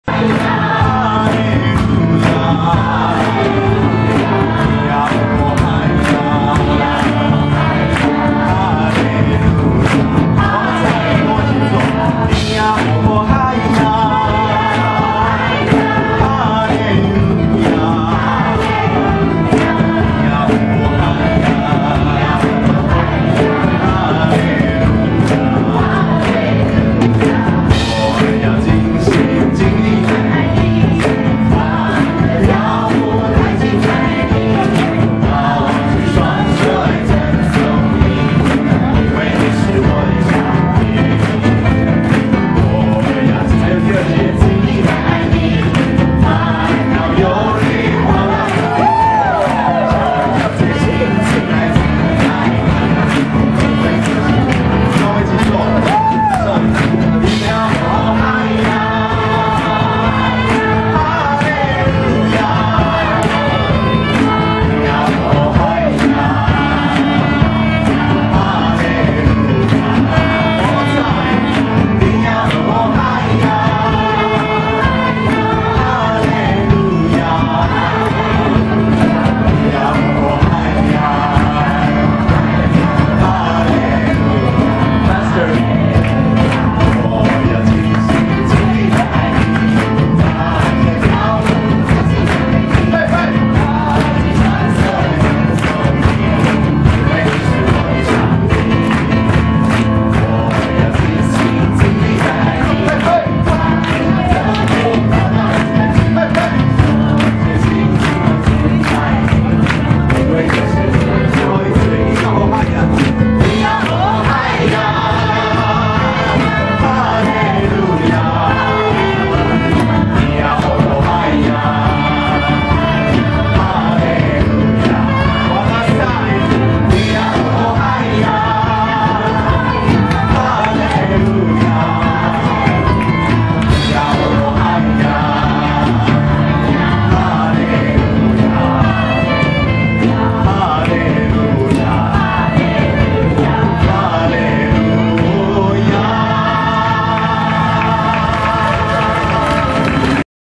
ORTVChapel 11-26 Aboriginal Song.MP4
At ORTV's Morning Chapel, November 26, 2009, we sang an aboriginal song. The native Taiwanese music sounds a lot like Native American music. Curious